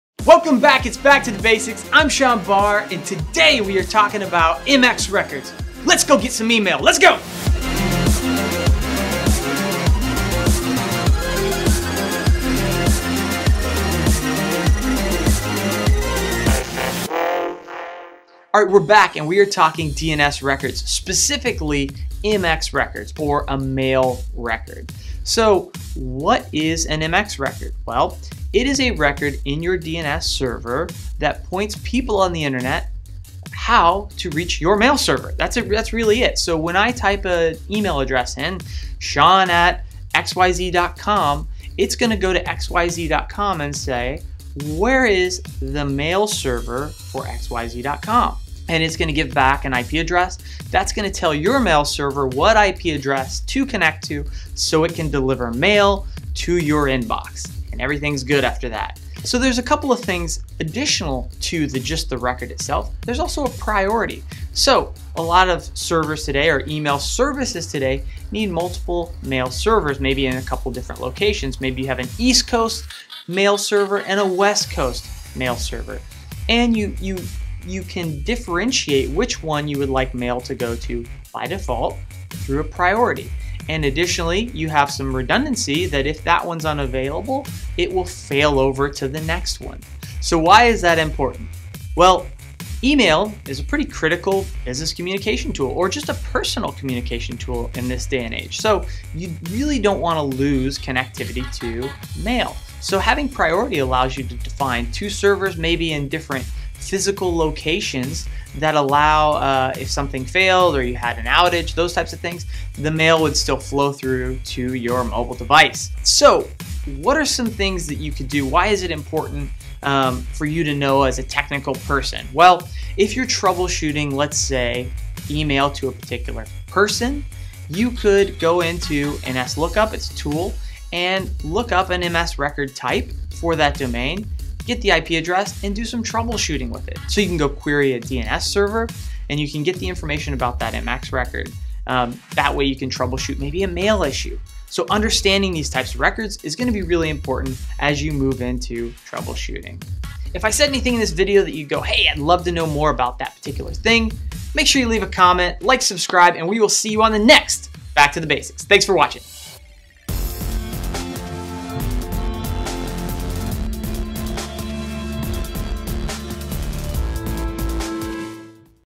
MX Records | DNS Records
MX, or mail, records are the records in your DNS server that point people on the Internet to how to reach your mail server!